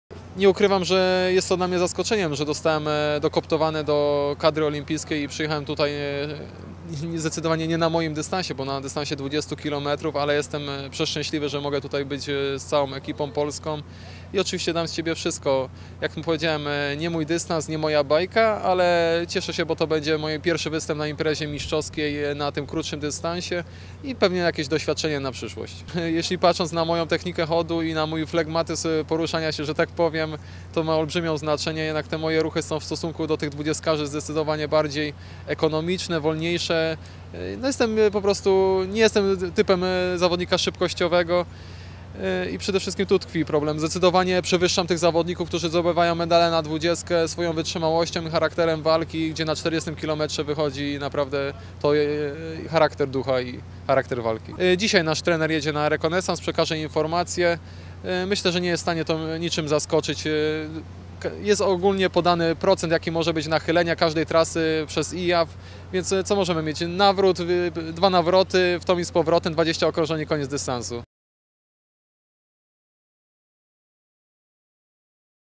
Łukasz Nowak
Chodziarz Łukasz Nowak przed startem na dystansie 20 km w igrzyskach olimpijskich w Rio de Janeiro